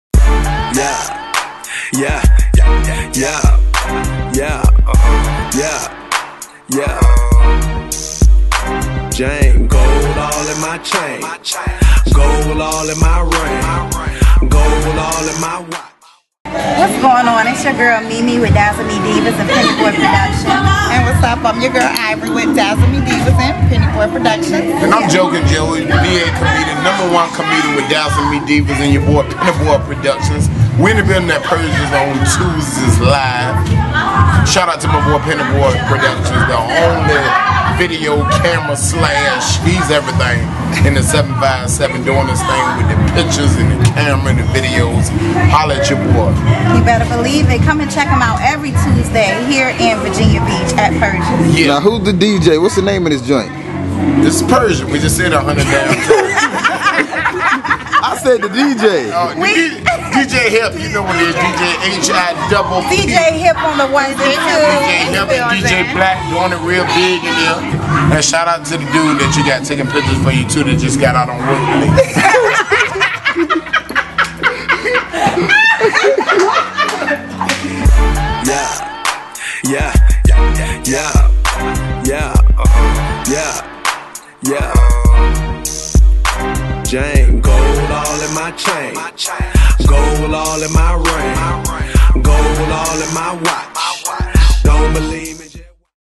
Takeover Tuesdays Promo Feat. Jokin Joe & DazzleMi Divaz - Penny Boi Productions
Every Tuesday join DJ Hipp & DJ Blaac at Purrsha Lounge as they GO IN on the tables mixing the best of your favorite Old School, New School, and Reggae jams! Happy Hour from 6p-9p with a $100 cash prize & photoshoot giveaway!!!  The party don't stop til Midnight and  Penny Boi Productions is in the building providing professional media coverage and snappin those crispy pics all night long. Special Guest - DazzleMi Divaz & Comedian Jokin Joe!